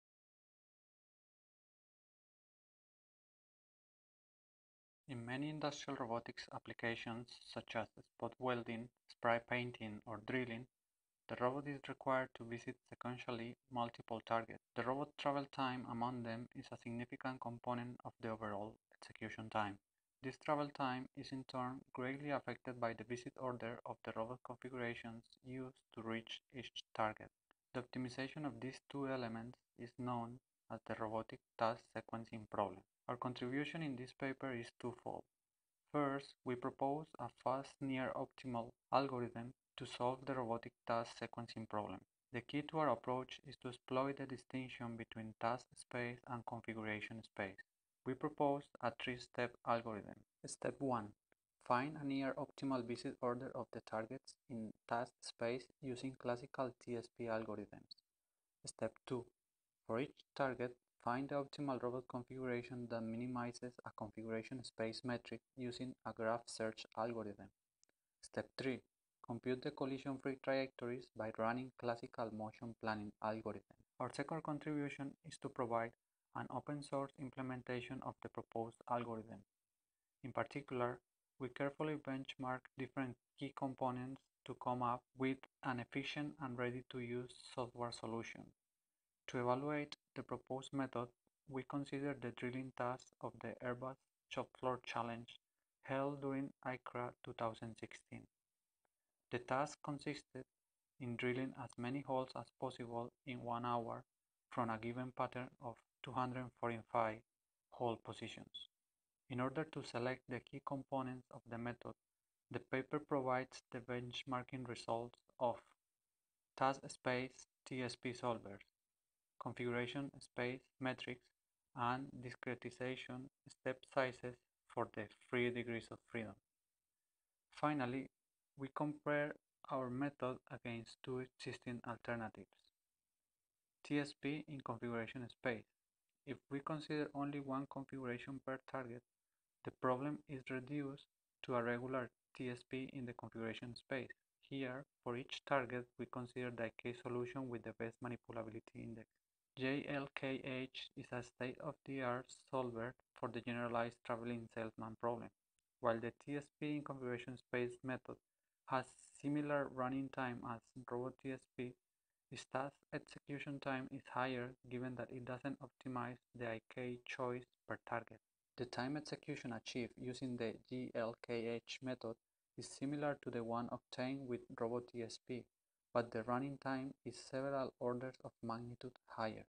RoboTSP - a Fast Solution to the Robotic Task Sequencing Problem
Interactive Session Tue PM Pod E.5
Authors: Suárez-Ruiz, Francisco; Lembono, Teguh Santoso; Pham, Quang-Cuong
Title: RoboTSP - a Fast Solution to the Robotic Task Sequencing Problem

Abstract:
In many industrial robotics applications, such as spot-welding, spray-painting or drilling, the robot is required to visit successively multiple targets. The robot travel time among the targets is a significant component of the overall execution time. This travel time is in turn greatly affected by the order of visit of the targets, and by the robot configurations used to reach each target. Therefore, it is crucial to optimize these two elements, a problem known in the literature as the Robotic Task Sequencing Problem (RTSP). Our contribution in this paper is two-fold. First, we propose a fast, near-optimal, algorithm to solve RTSP. The key to our approach is to exploit the classical distinction between task space and configuration space, which, surprisingly, has been so far overlooked in the RTSP literature. Second, we provide an open-source implementation of the above algorithm, which has been carefully benchmarked to yield an efficient, ready-to-use, software solution. We discuss the relationship between RTSP and other Traveling Salesman Problem (TSP) variants, such as the Generalized Traveling Salesman Problem (GTSP), and show experimentally that our method finds motion sequences of the same quality but using several orders of magnitude less computation time than existing approaches.